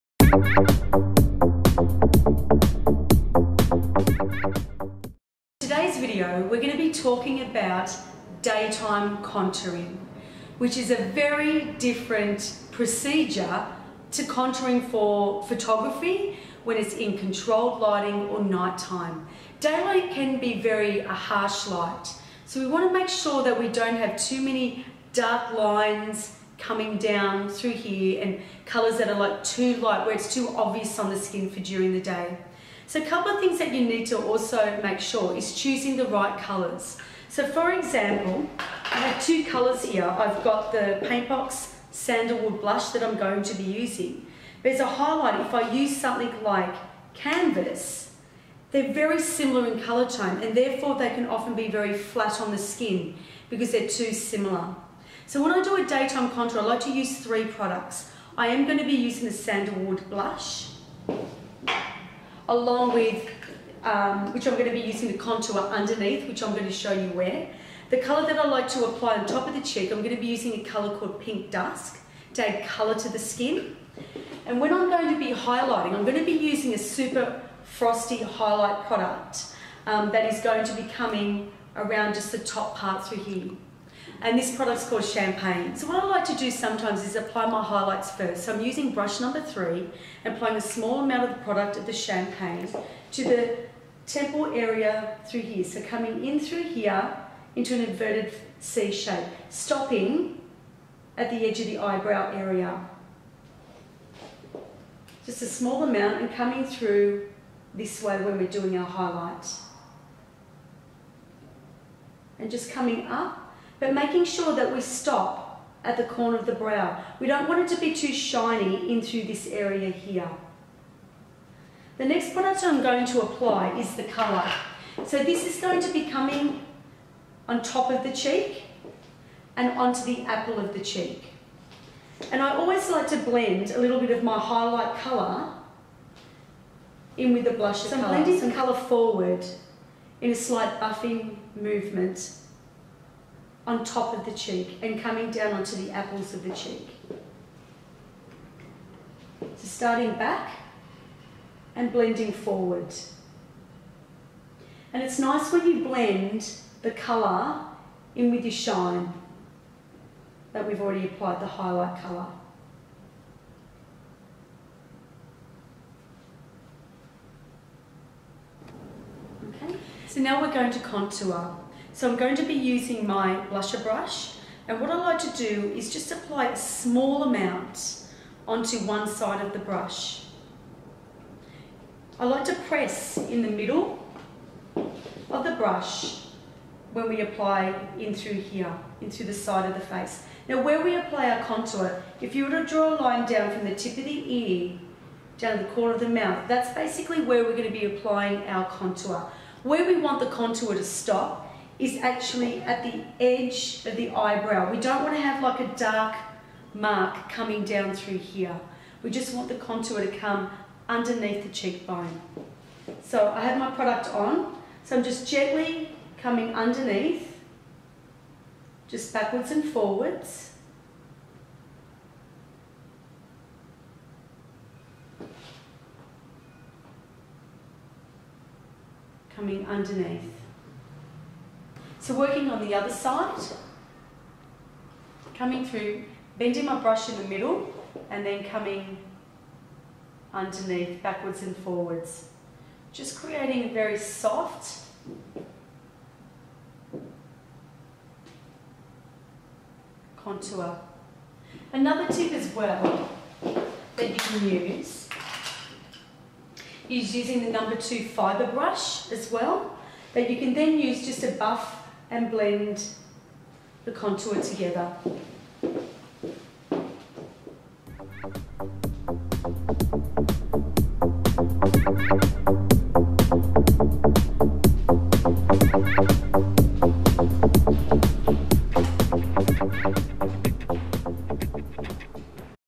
Daytime Contouring With PaintBox Cosmetics
Follow these Steps to Master daytime Contouring.  The Products used are:
Highlighter- Champagne
Cheek- Pink Dusk
Contour- Sandalwood
One all 3 Products have been applied Buff with the #2 Stippling Brush